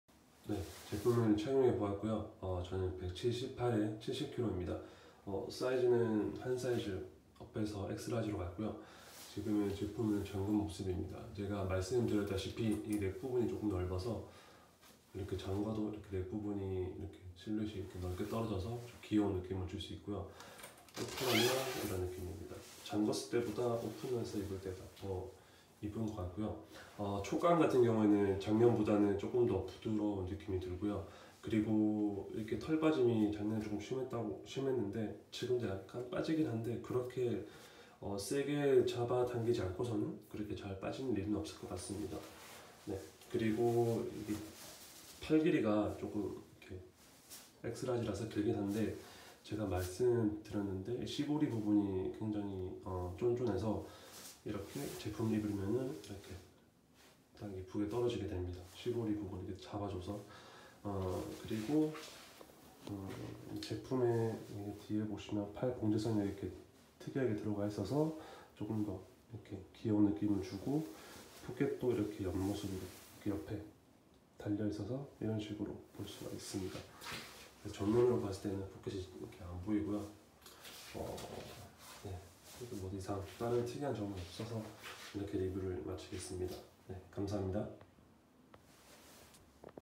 [이 시국에 유니클로?] 19fw 유니클로 u 후리스 착용 샷!
U르메르 후리스 제품 착용입니다.
실측은 넥 8.5   /어깨 51 (봉제선 끝과 끝)   /기장72.5(넥부분 제외,뒷 기장)    /가슴 64.5    / 팔 기장 62.5